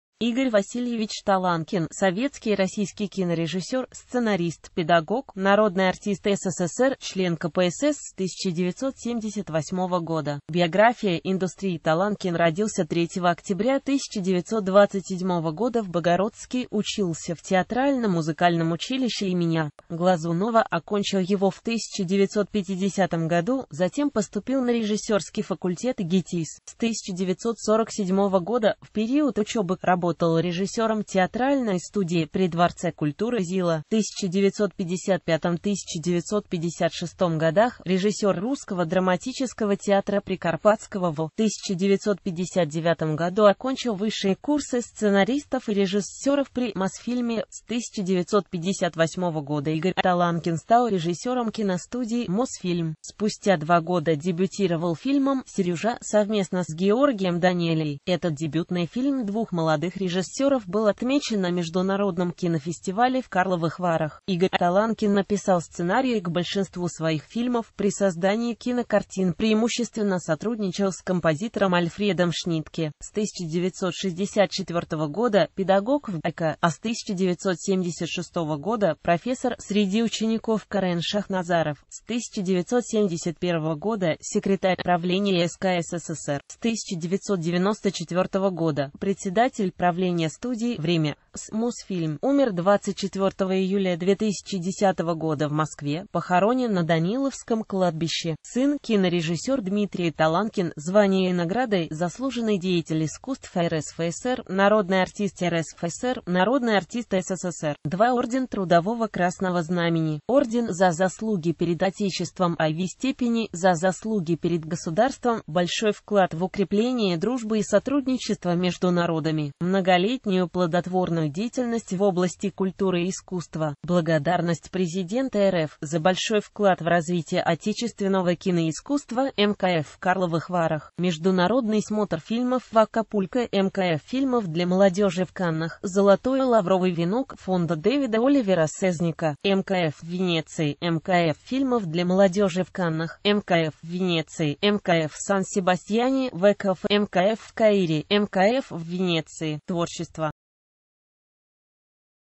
Таланкин, Игорь Васильевич
И́горь Васи́льевич Тала́нкин — советский и российский кинорежиссёр, сценарист, педагог.Народный артист СССР .Член КПСС с 1978 года.